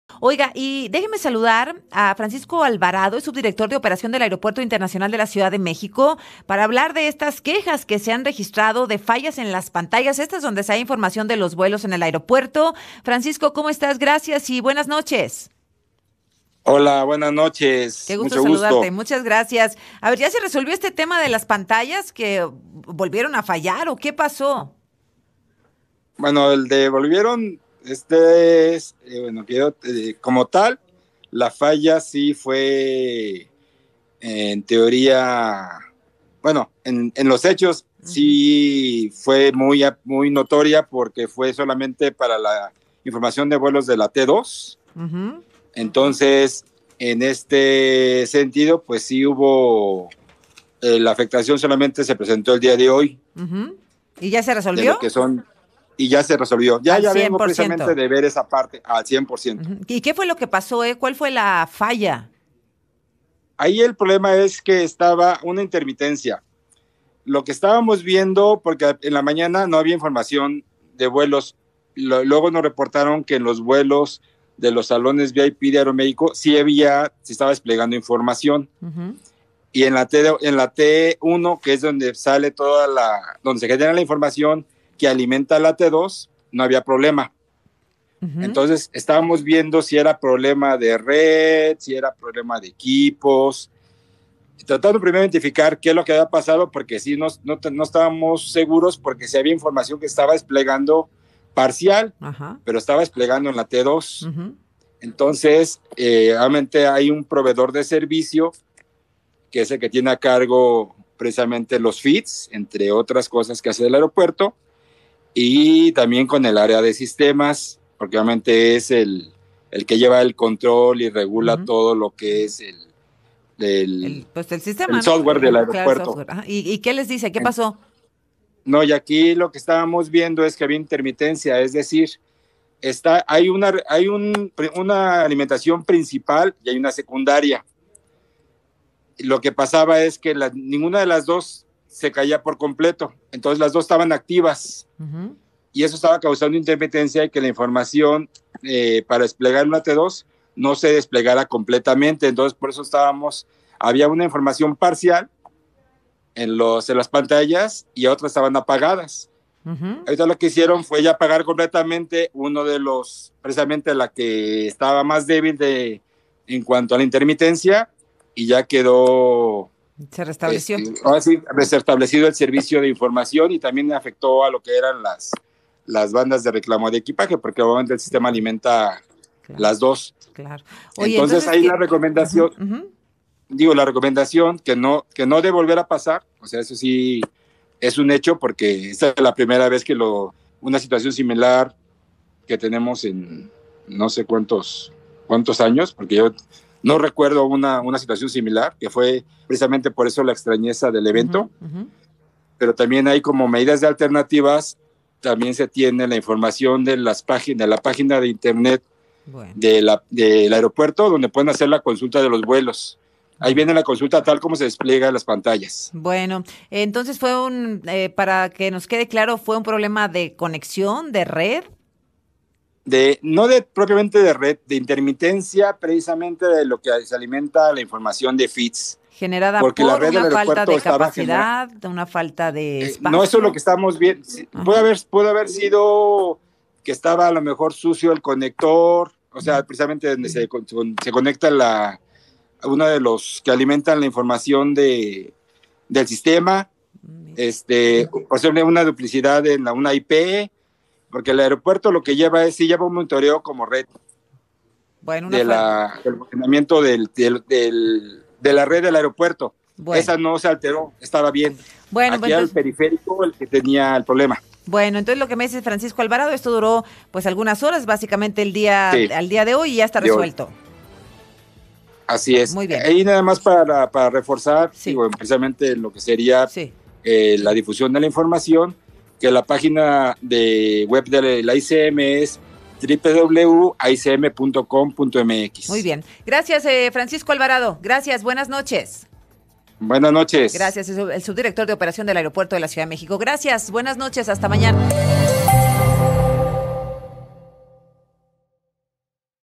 ¿Por qué fallaron las pantallas en la terminal 2 del AICM?
AzucenaxFórmula | Francisco Alvarado, subdirector del AICM, indicó que las pantallas de los vuelos volvieron a fallar y en los hechos sí fue muy notoria porque fue sólo para vuelos de la terminal 2, pero ya fue reparado al 100%